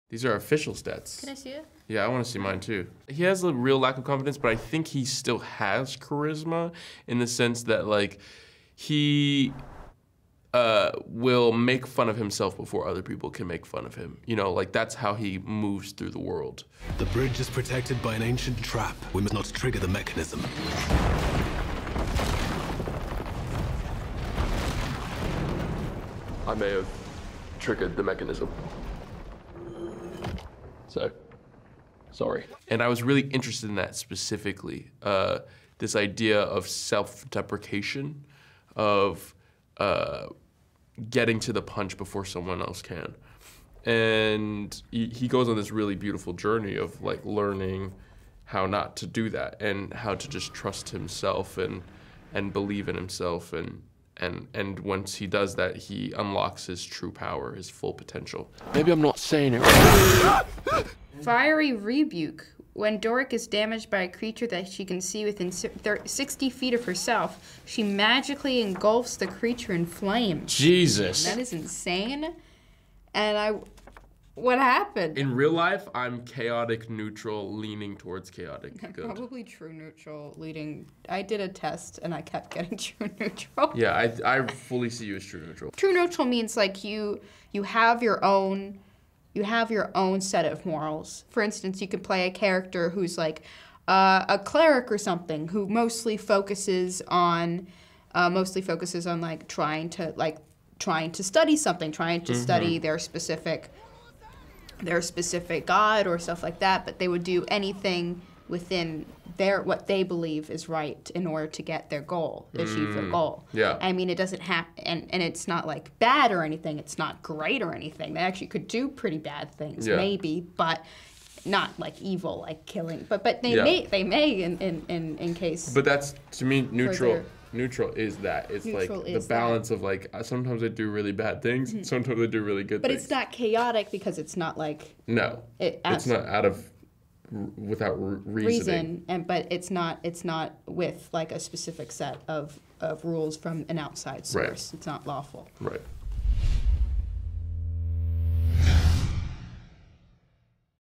D&D : l'interview Feuille de Perso avec Justice Smith et Sophia Lillis !
Le film Donjons & Dragons L'Honneur des voleurs sort le 12 avril en France, on a pu confronter Justice Smith (Simon) et Sophia Lillis (Doric) à leurs feuilles de perso officielles tirées du C'est parti en vrille très vite.